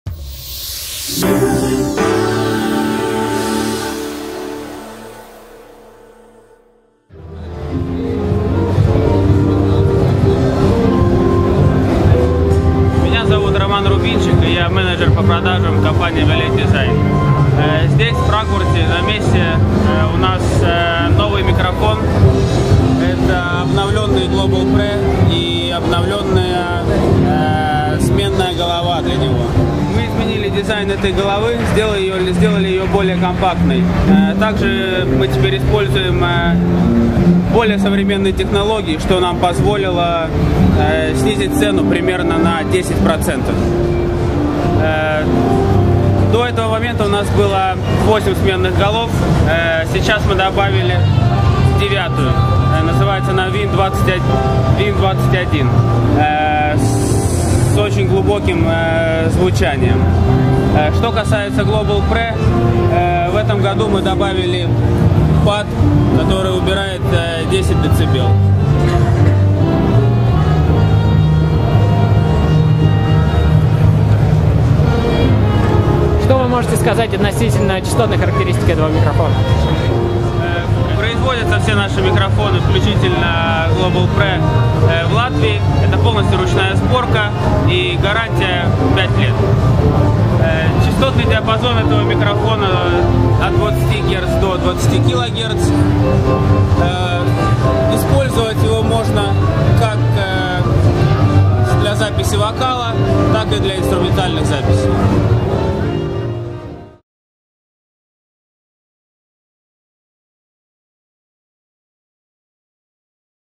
mmag.ru: Musikmesse 2011 - Violet Design Global Pre plus new VIN21 head
(MusicMag) представляет видео-репортаж с выставки Musikmesse-2011 о новом микрофоне Violet Design Global Pre с новой сменной головой VIN21.